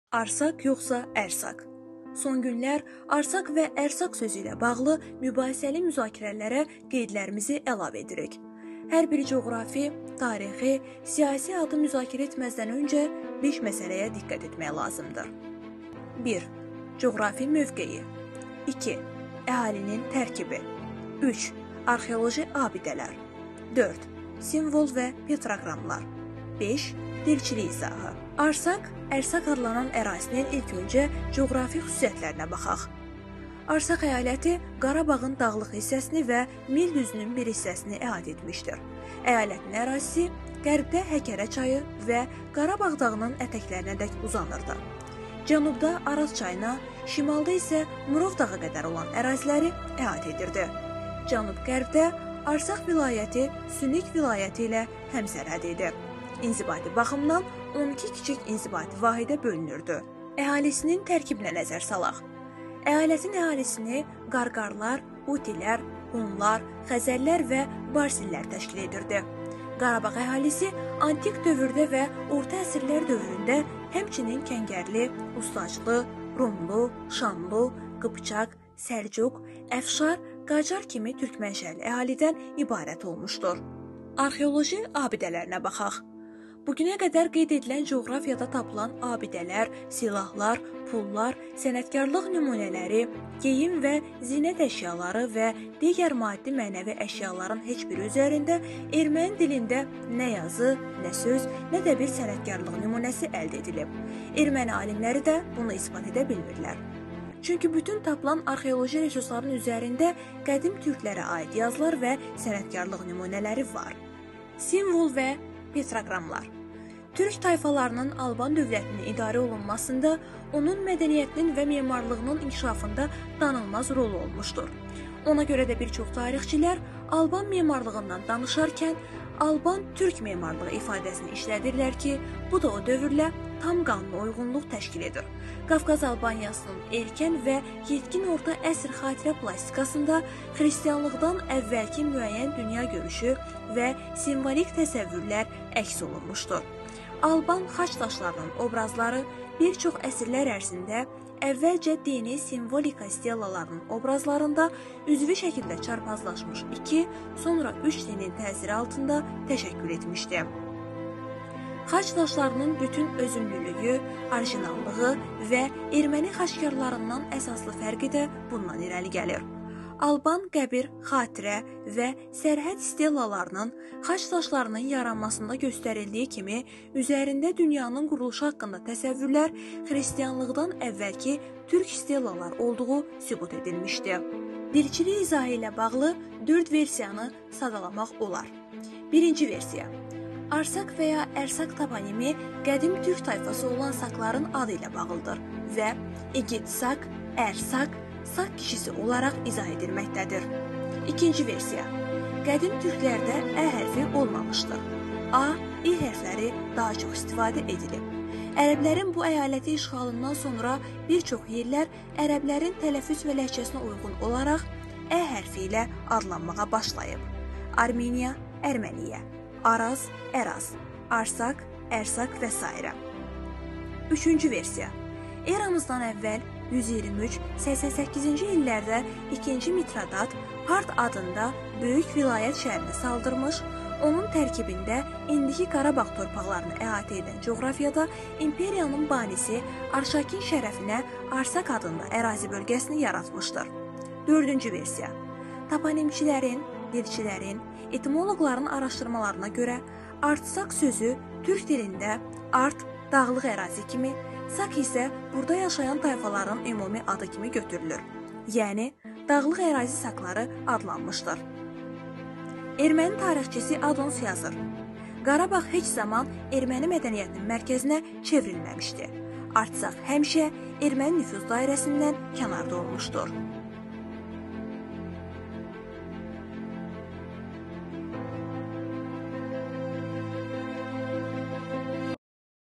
ARSAK, yoxsa ƏRSAK?!
II Qarabağ savaşının getdiyi bir zamanda Qarabağın əski adı olan Arsak haqqında yaranmış mübahisəli məqama aydınlıq gətirməyi özümüzə borc bildik. Mübahisəli məsələ ilə bağlı müxtəlif elmi yazılar işıqlandırılmışdır.

Bununla bağlı hazırladığımız videonu diqqətinizə çatdırmaq istəyirik. Bizi izləməyi unutmayın.